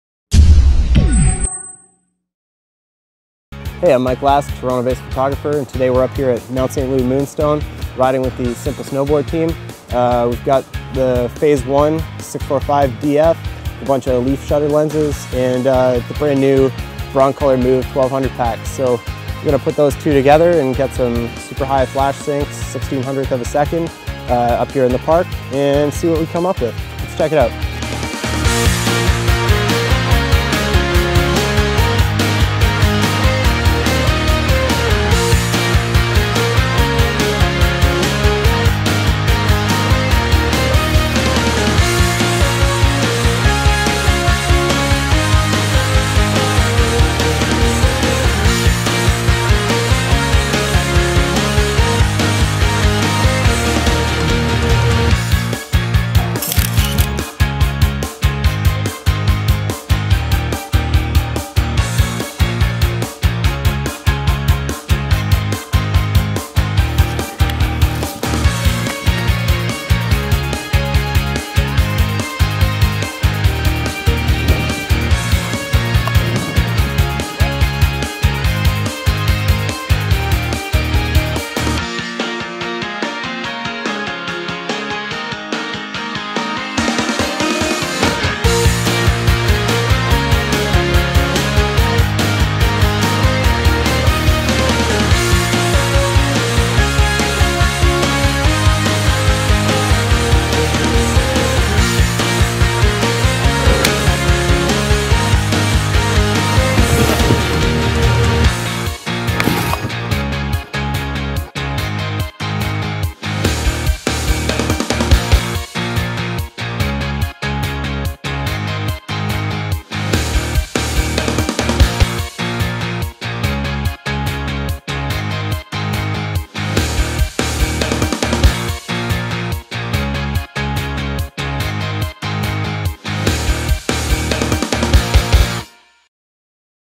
Broncolor Move 1200 | Simple Snowboarding | High Speed Flash Sync
In February 2013, Broncolor Canada invited us to test out the latest Broncolor Move 1200L flash system. With the Phase One 645DF and IQ160, we were able to achieve 1/1600 of a second flash sync speeds and this video features some of those results.